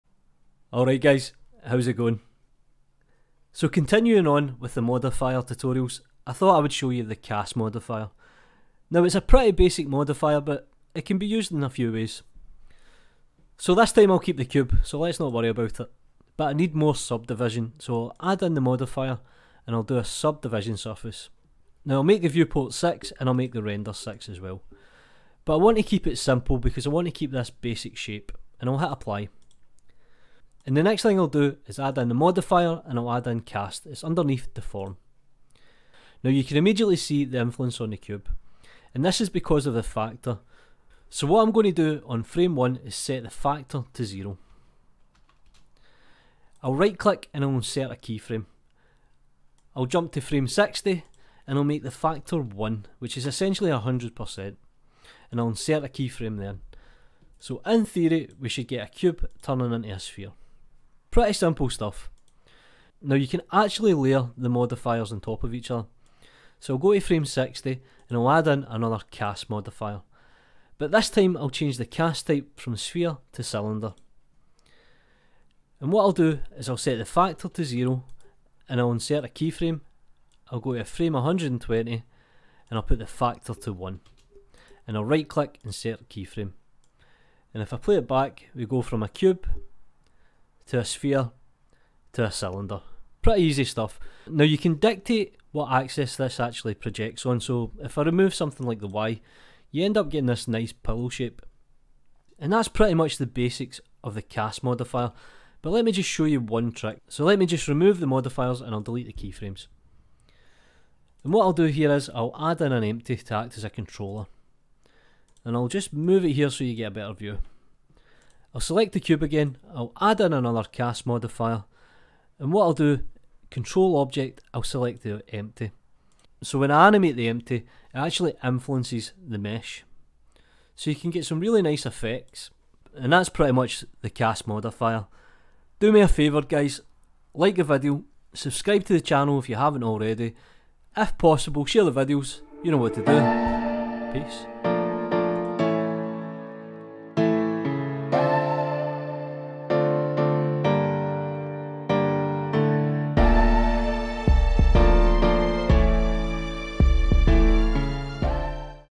Blender 2.8 Tutorial: Cast Modifier
Use Blenders cast modifier to blend between shapes.

The Cast modifier shifts the shape of a mesh, curve, surface or lattice, towards any of a few predefined shapes (sphere, cylinder, cuboid).

It is equivalent to the To Sphere tool in Edit Mode, and what other programs call “Spherify” or “Spherize”, but, as written above, it is not limited to casting to a sphere.